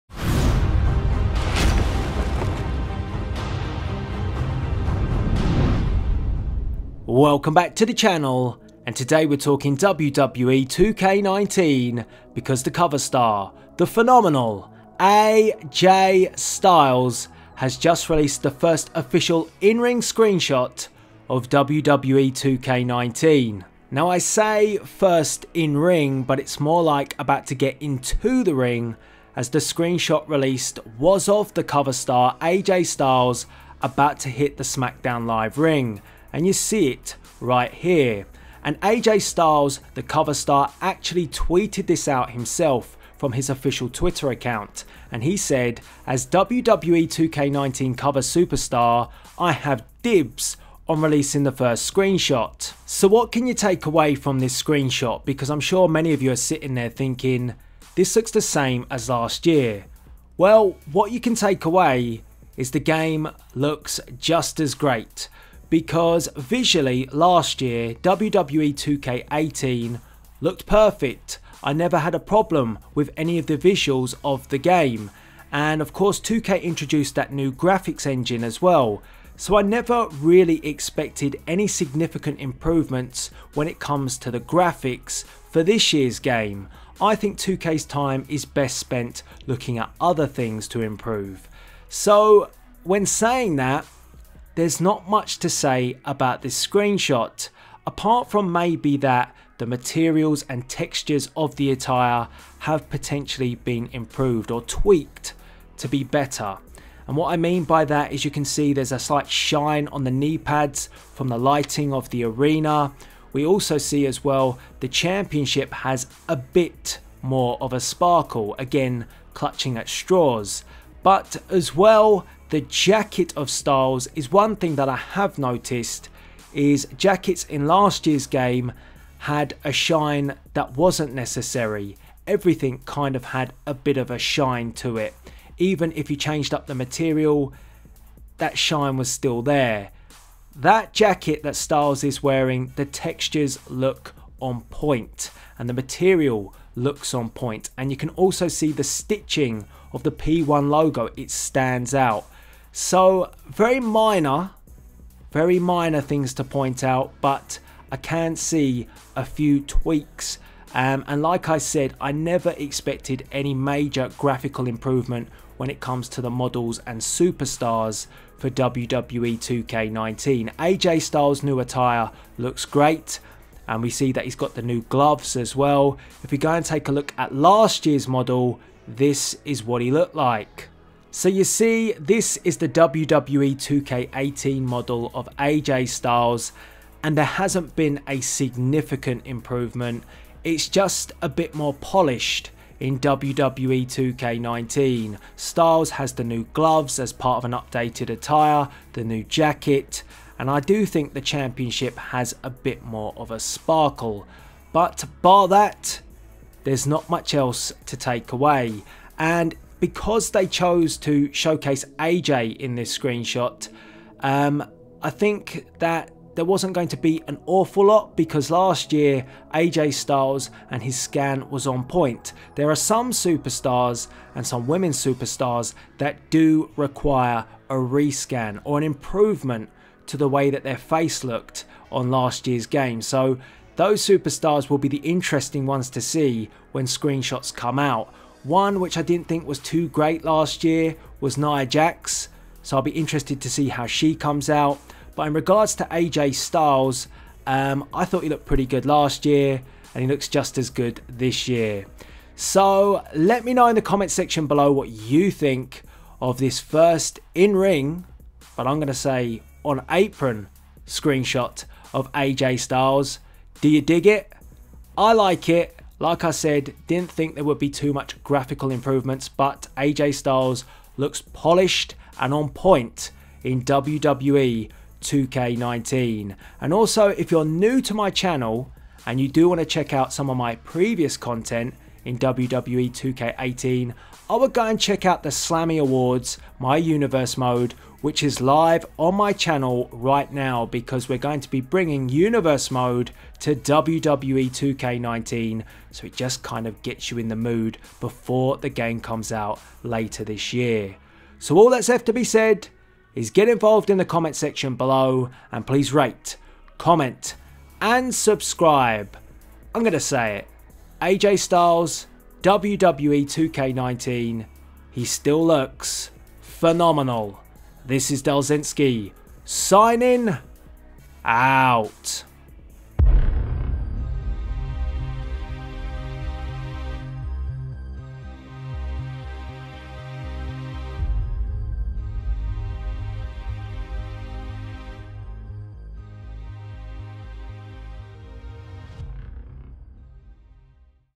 WWE 2K19 1st Screenshot Revealed By AJ Styles + Comparison From Last Year 🤔
First (about to get) in ring shot of AJ Styles for WWE 2K19 has been revealed. Let discuss the improvements and compare to last years game.

Let me know what you think via the comments below!